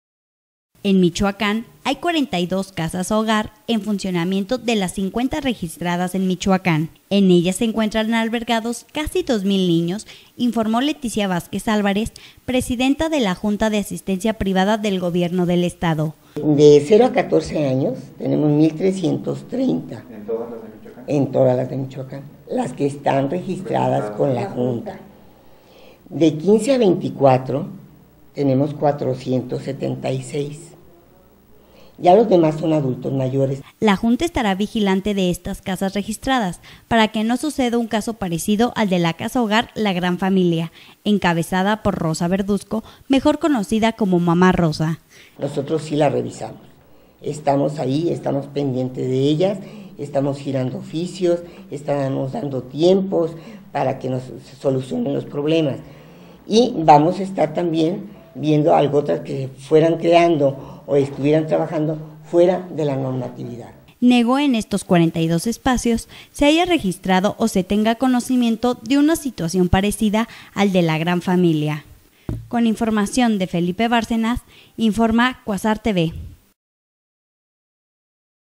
Hay dos mil niños en Casa Hogar de Michoacán
En Michoacán hay 42 Casas Hogar en funcionamiento de las 50 registradas en Michoacán...